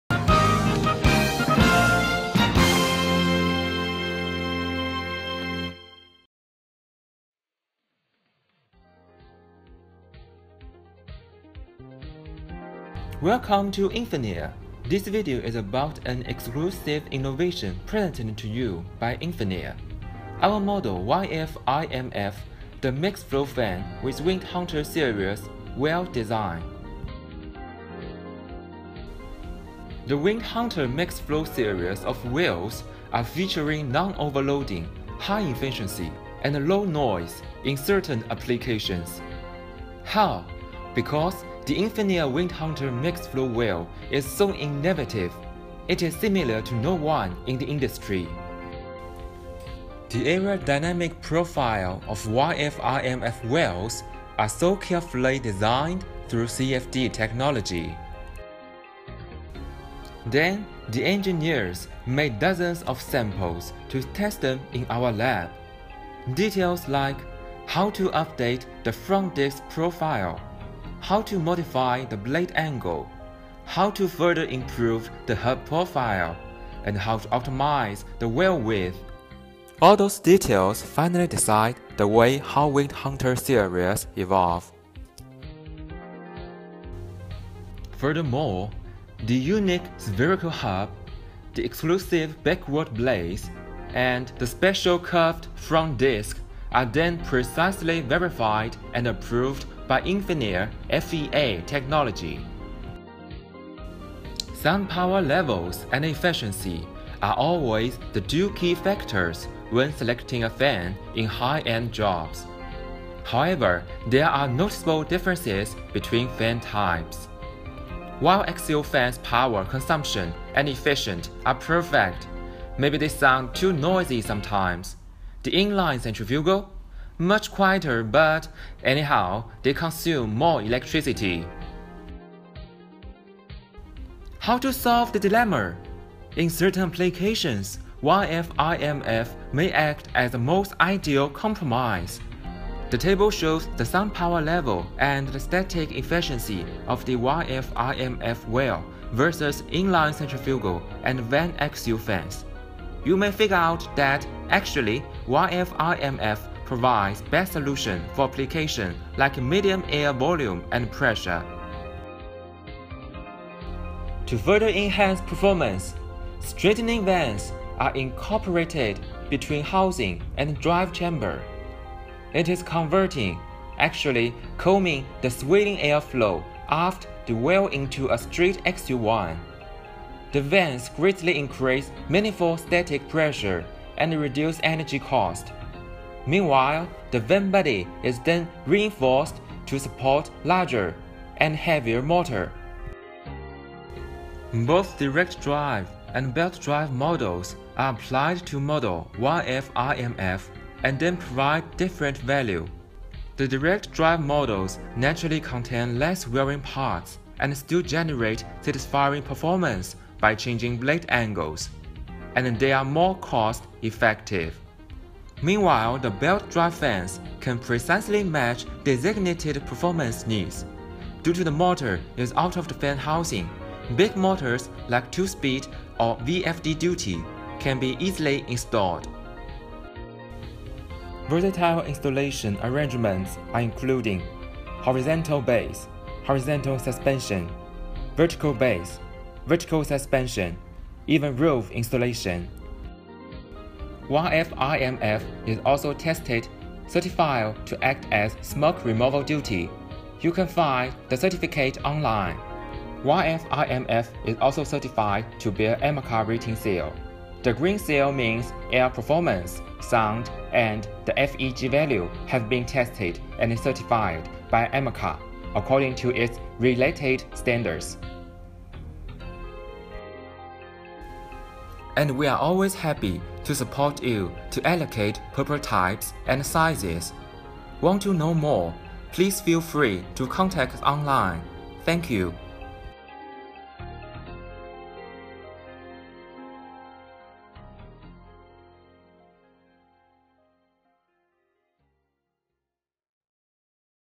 Inline Mixed-flow fan – Model YFIMF
This video is about an exclusive innovation presented to you by INFINAIR, our model YFIMF, the mixed flow fan with Wind-Hunter series wheel design. The Wind-Hunter mixed flow series of wheels are featuring non-overloading, high efficiency and low noise in certain applications. Through contrast with inline centrifugal fan and axial fan, YFIMF provides best solution for application like medium air volume and pressure. YFIMF is also certified to bear AMCA (Air Movement and Control Association) certified rating seal.